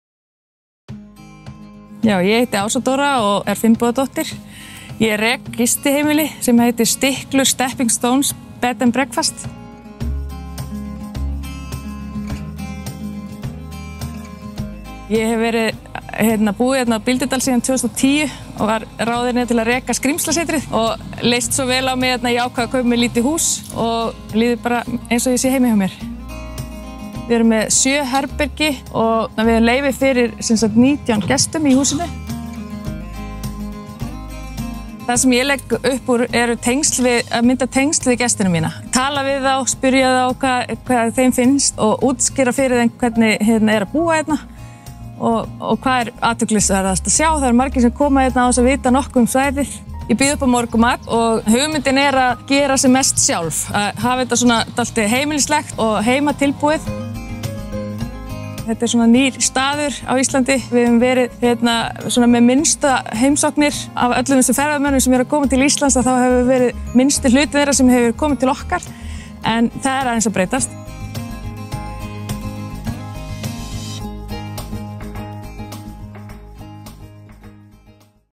Bíldudalur - Stiklur, Steppin´stones Bed&Breakfast
This family-owned guesthouse is located in Bíldudalur village in Iceland’s Westfjords region. It offers brightly decorated rooms with free Wi-Fi access and mountain or fjord views.